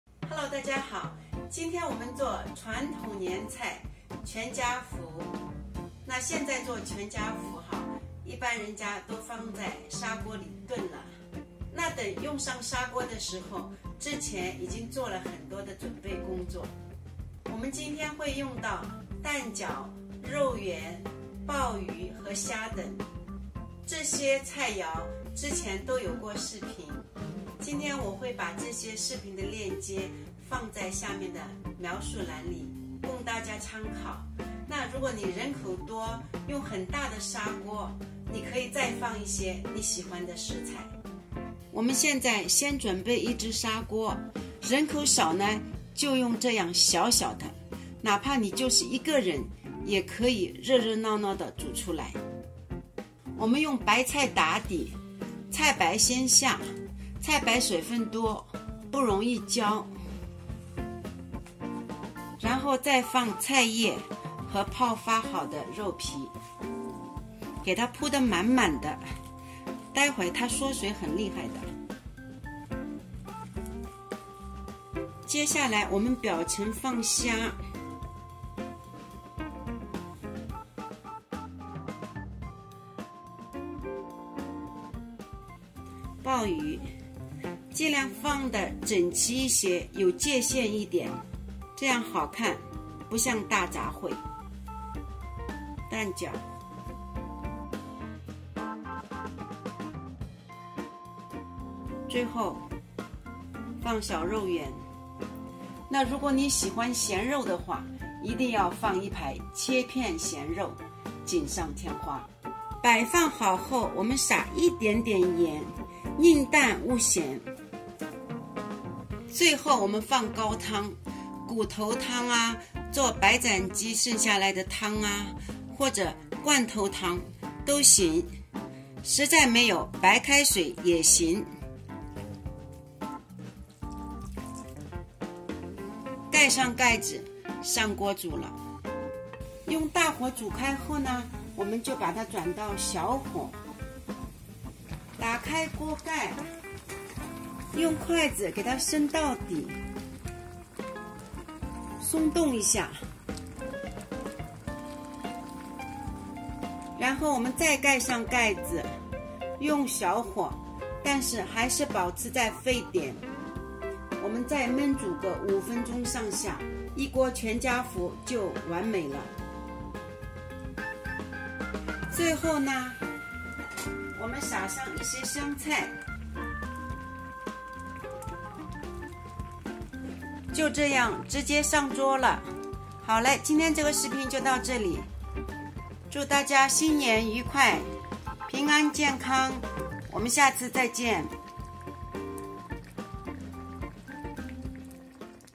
传统年菜：全家福！ Chinese New Year Stewed Assorted Delicacies (Quan Jia Fu)!
Welcome to Jenny's Channel! Here, Jenny shares her recipes for Chinese cuisine and many other dishes! Today, a recipe for Quan Jia Fu! A stewed pot filled with cabbage and foods meant to celebrate Chinese New Year, add your own twist to make it even more special.

狮子头(菜谱一样，把肉丸弄小) | Lion's Head Meatballs (Same recipe is used in the video, but made smaller)